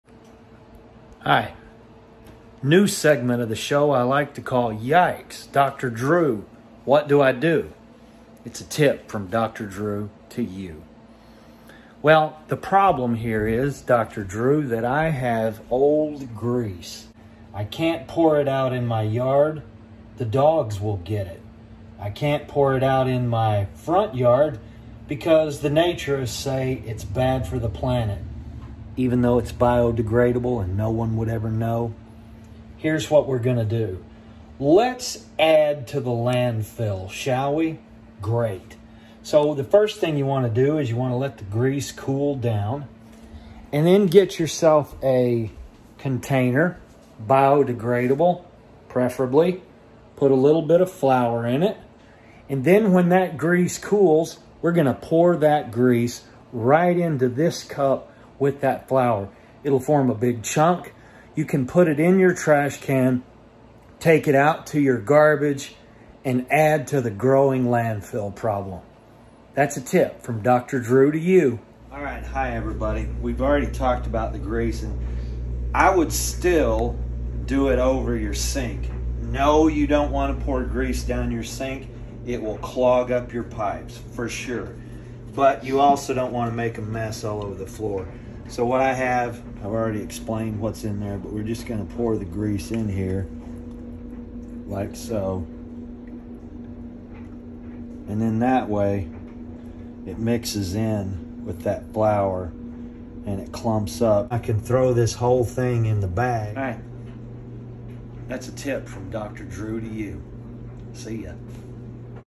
Yikes! Dr. Drew what do I do?
A tip from Dr. Drew to you: what do I do with used grease?